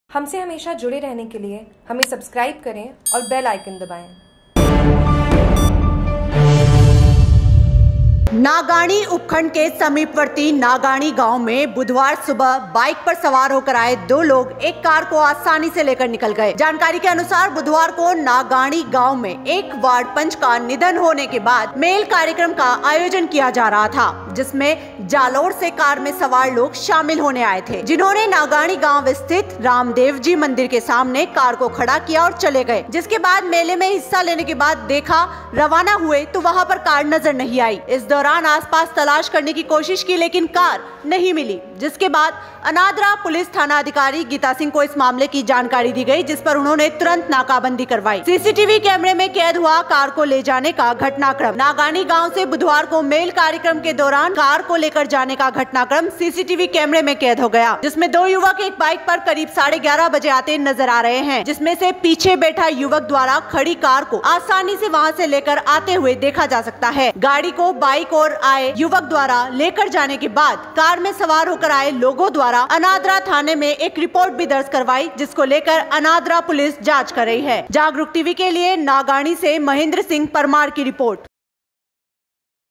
नागाणी : बुधवार सुबह बाईक पर सवार होकर आए दो चोरो ने एक कार को आसनी से लेकर निकल गए
उपखंड के समीपवर्ती नागाणी गाँव में बुधवार सुबह बाईक पर सवार होकर आए दो लोगों एक कार को आसनी से लेकर निकल गए। जानकारी के अनुसार बुधवार को नागाणी गाँव में एक वार्डपंच का निधन होने के बाद मेल कार्यक्रम का आयोजन किया जा रहा था जिसमें जालौर से कार में सवार लोग शामिल होने आए थे। जिन्होंने नागाणी गाँव स्थित रामदेवजी मंदिर के सामने कार को खडा कर चले  गए। जिसके बाद मेल में हिस्सा लेने के बाद देखा रवाना हुए तो वहाँ पर कार नही दिखी ओर इस दौरान आसपास तलाश करने की कोशिश की लेकिन कार नहीं मिली। जिसके बाद अनादरा पुलिस थानाधिकारी गीतासिंह को इस मामले की जानकारी दी जिस पर तुरन्त ही नाकाबंदी करवाई गई।

सीसीटीवी कैमरे में केद हुआ कार को ले जाने का घटनाक्रम

नागाणी गाँव से बुधवार को मेल कार्यक्रम के दौरान कार को लेकर जाने का घटनाक्रम सीसीटीवी कैमरों में केद हो गया। जिसमें दो युवक एक बाईक पर करीब साढे ग्यारह बजे आते नजर आ रहे है। जिसमें से पीछे बैठे युवक द्वारा खडी कार को आसानी से वहाँ से लेकर आते हुए देखा जा सकता है। गाडी को बाईक ओर आए युवक द्वारा लेकर जाने के बाद कार में सवार होकर आए लोगों द्वारा अनादरा थाने में एक रिपोर्ट भी दर्ज करवाई जिसको लेकर अनादरा पुलिस जाँच कर रही है।